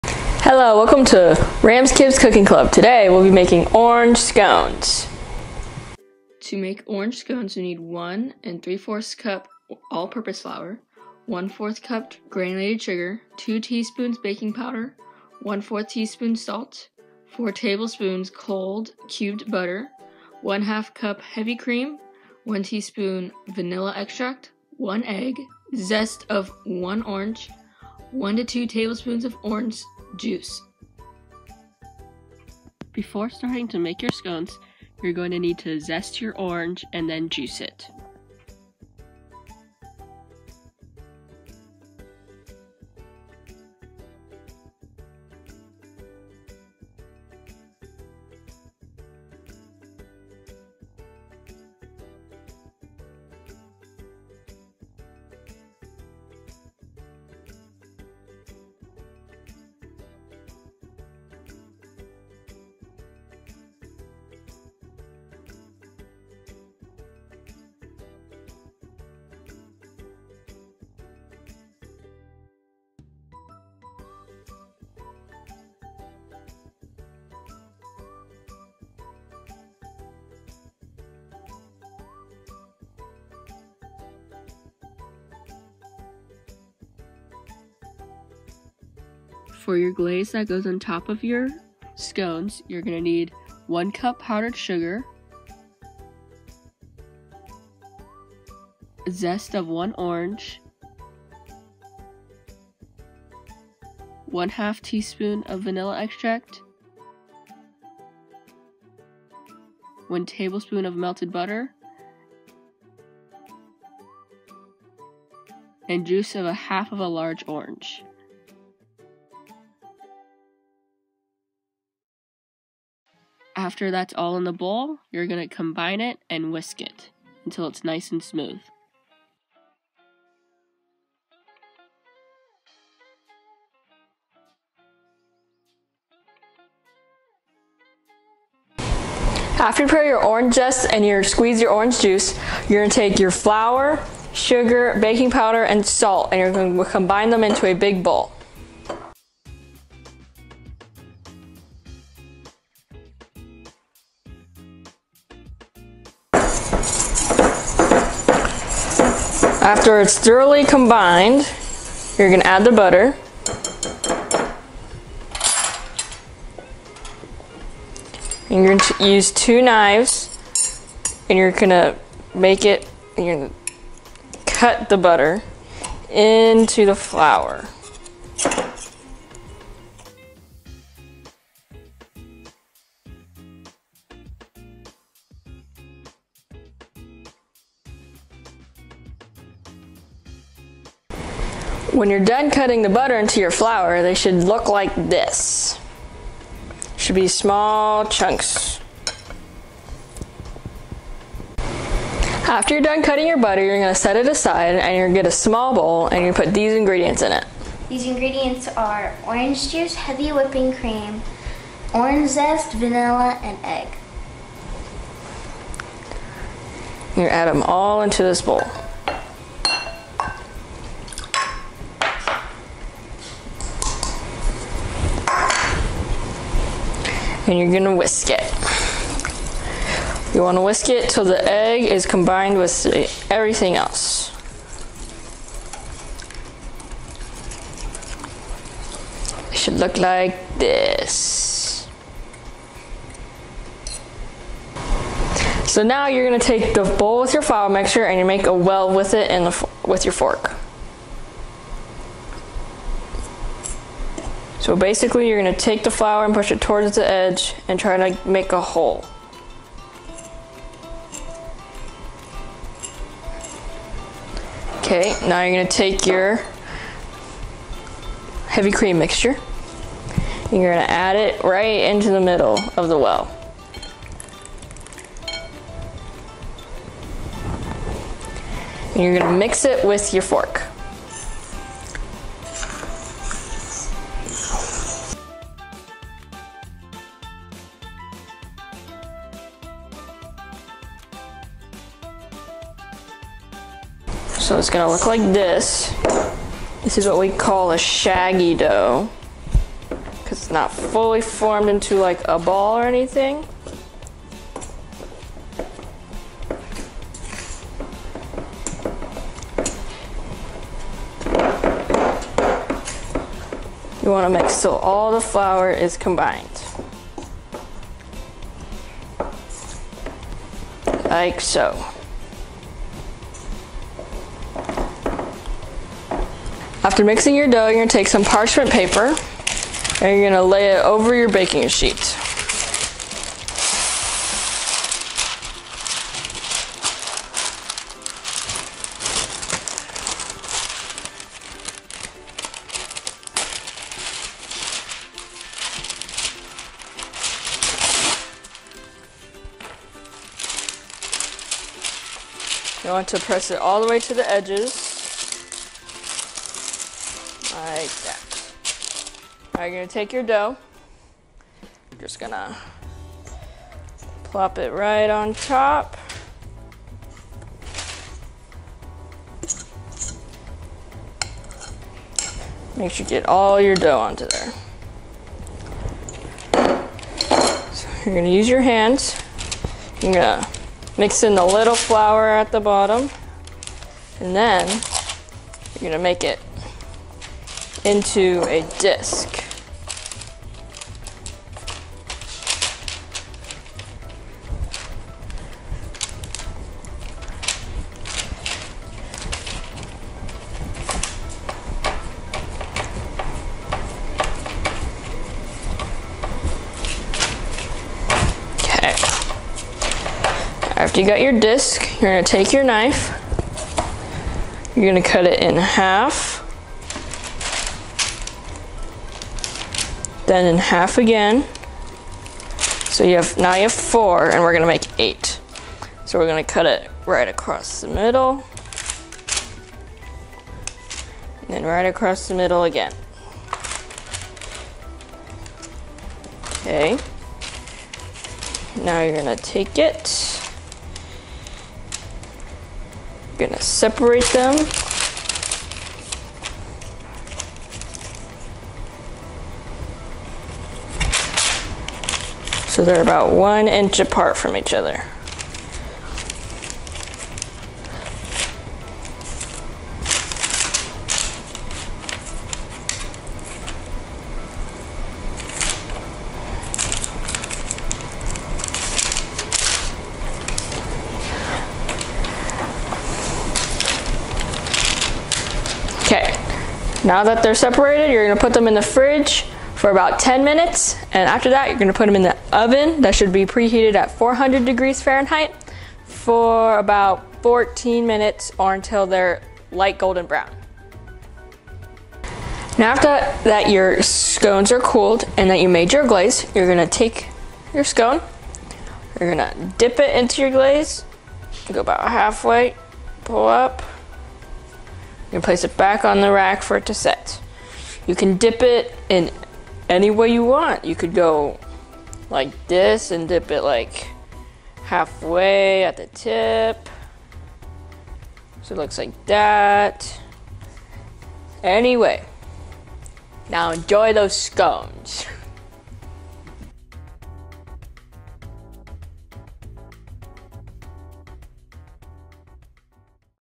Orange Scones Tutorial
Look at you making something so fancy for breakfast!  These orange scones are a fresh and delicious alternative to cereal or pancakes in the morning.  Join us as we learn to zest citrus fruit to bring these scrumptious scones to life.  They don't disappoint!
Please fill out this survey when you are finished to see how much you have learned!!
Survey:
Recipe By: The Fresh Cooky

Follow us on:
Editing: Rylee Freitag